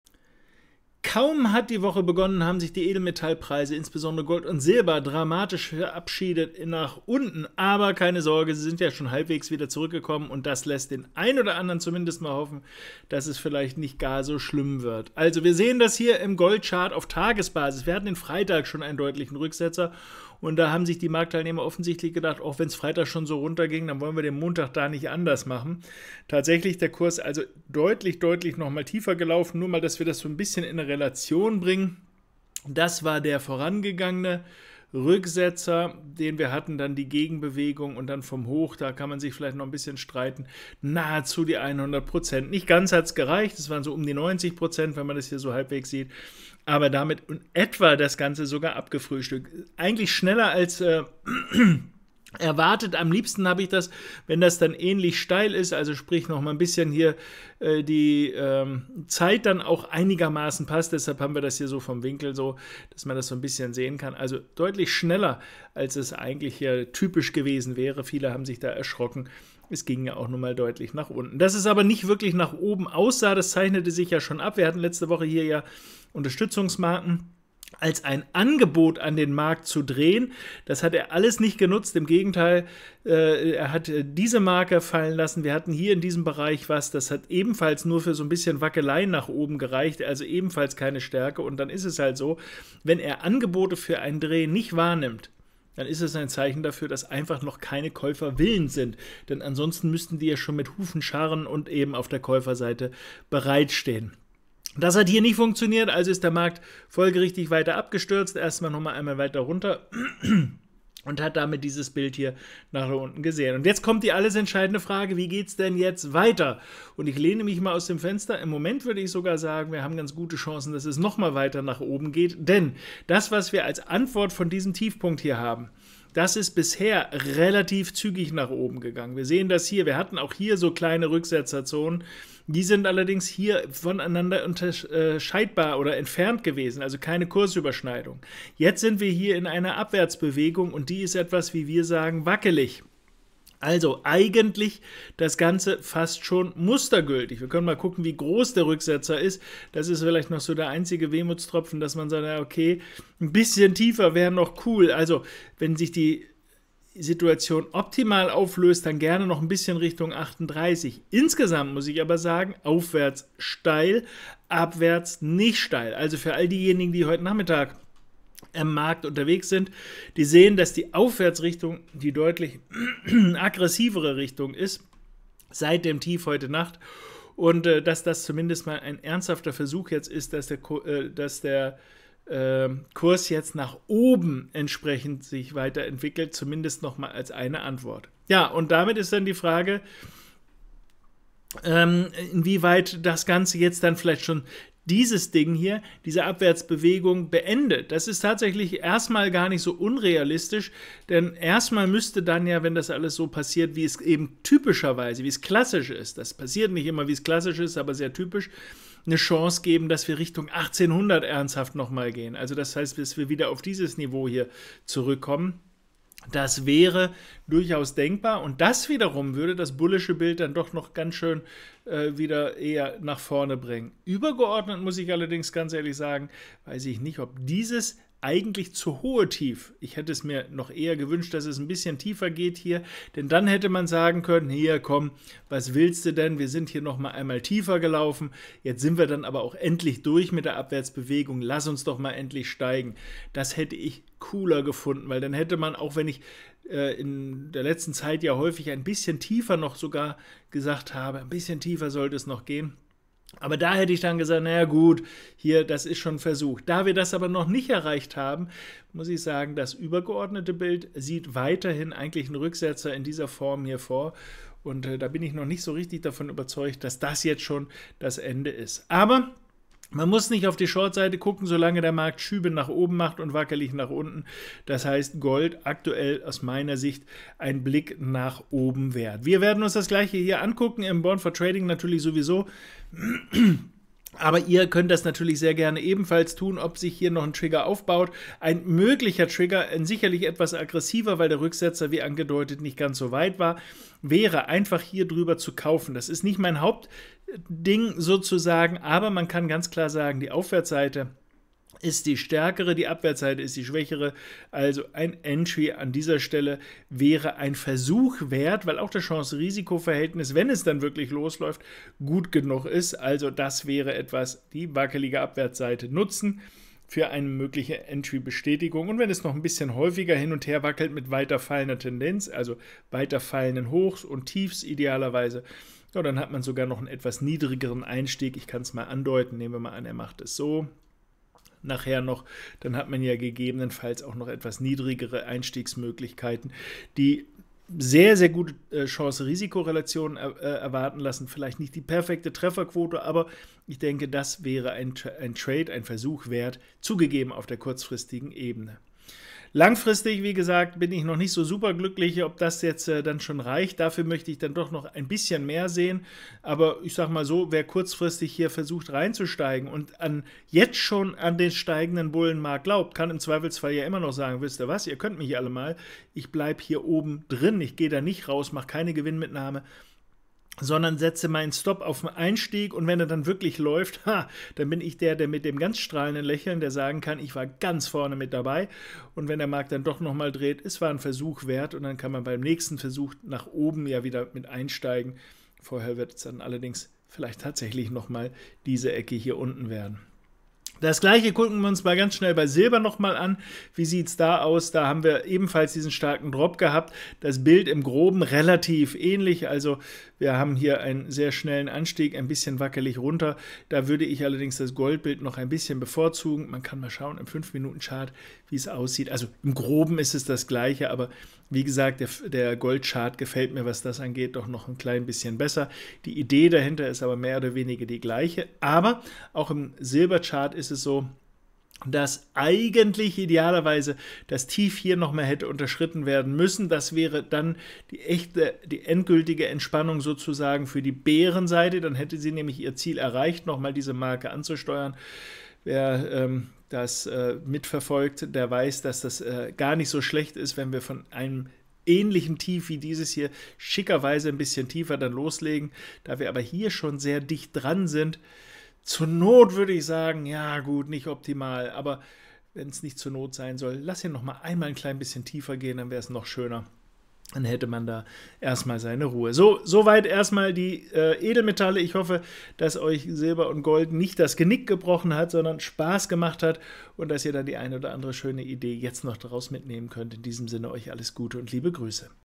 Rüdiger Born über den Flash Crash bei Gold - Anstieg voraus?
Gold ist heute früh kräftig abgestürzt und schnell wieder gestiegen. Ein Anzeichen für weitere Anstiege? Schauen wir im Video auf die Charts.